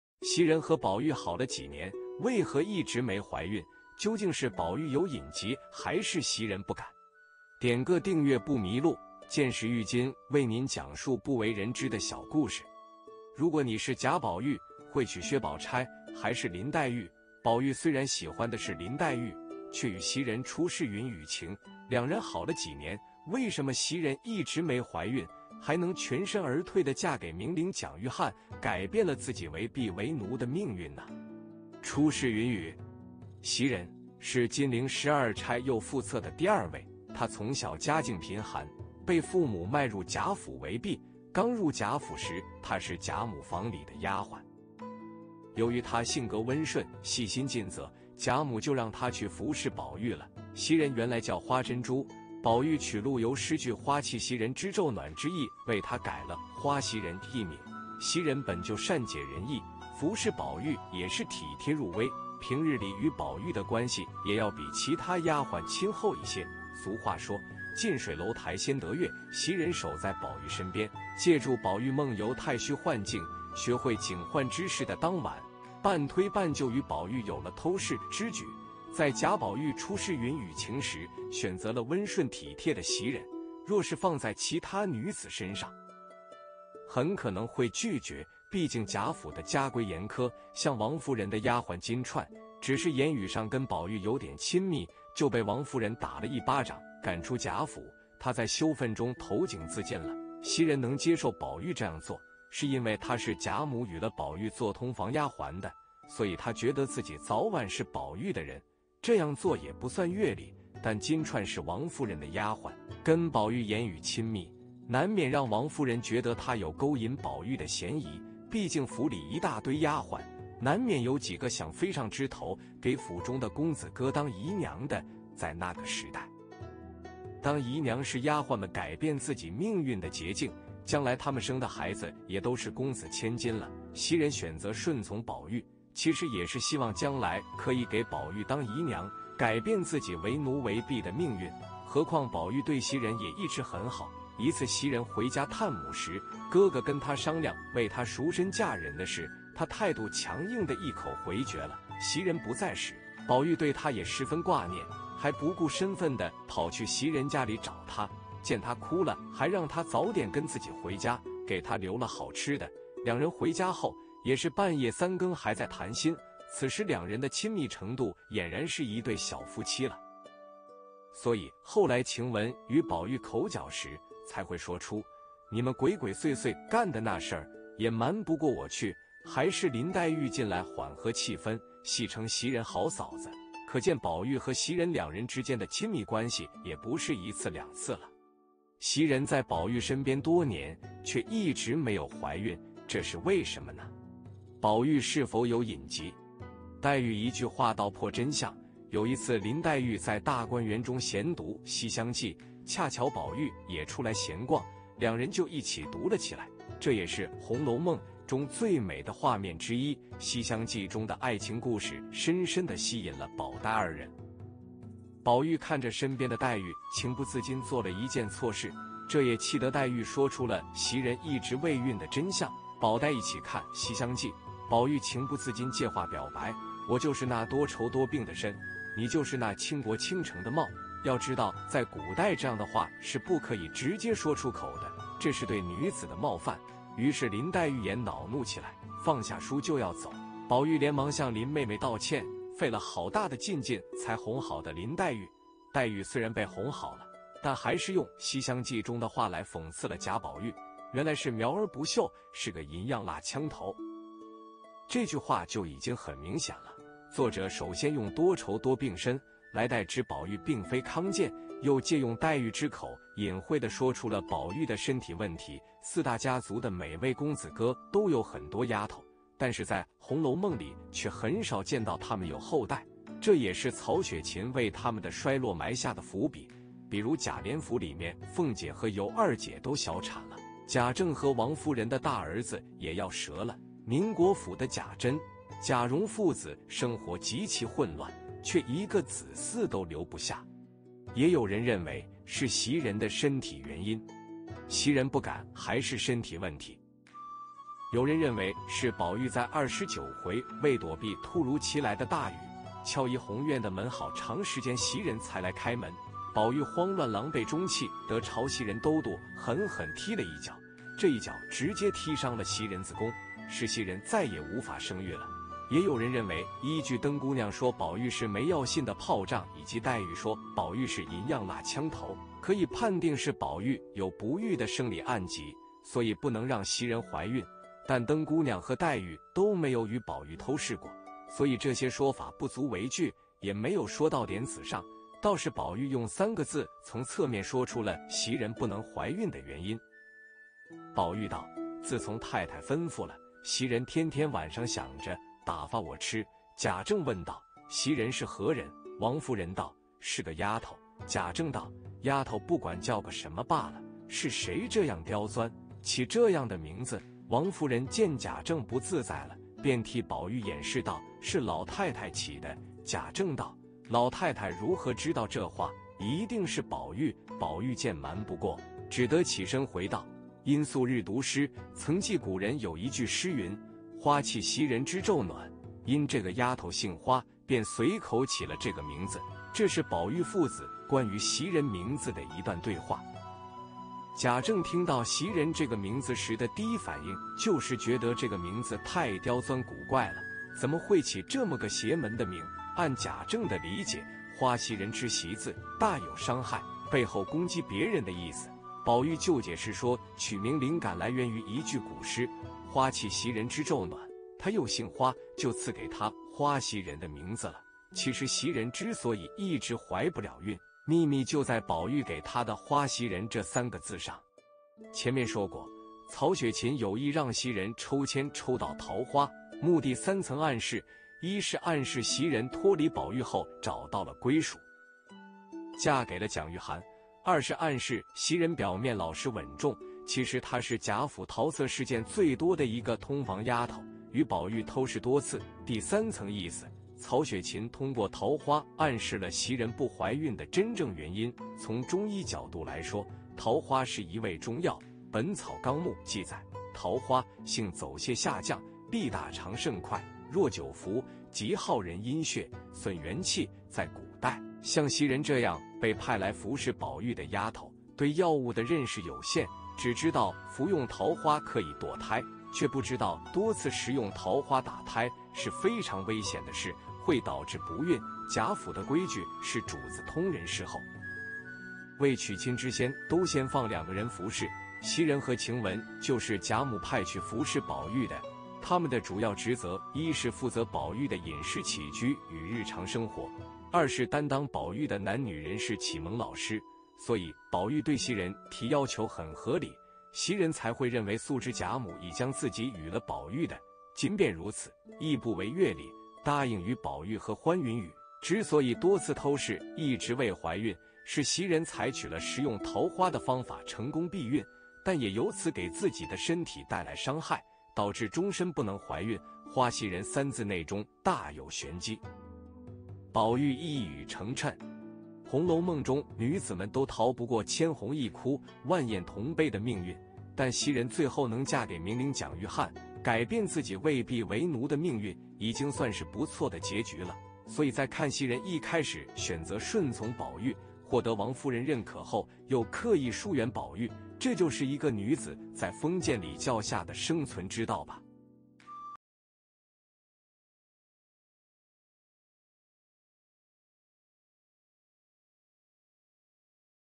襲人和寶玉好了幾年，為何一直沒懷孕？究竟是寶玉有隱疾還是襲人不敢？【鑒史禦今】歷史
如果你是賈寶玉，會娶薛寶釵，還是林黛玉？寶玉雖然喜歡的是林黛玉，卻與襲人 「初試雲雨情」，兩人好了幾年，為什麼襲人一直沒懷孕？還能全身而退地嫁給名伶蔣玉菡，改變了自己為婢為奴的命運呢？ 請注意，本影片是專注於講解社會新聞和奇聞異事，完全符合YouTube社群準則。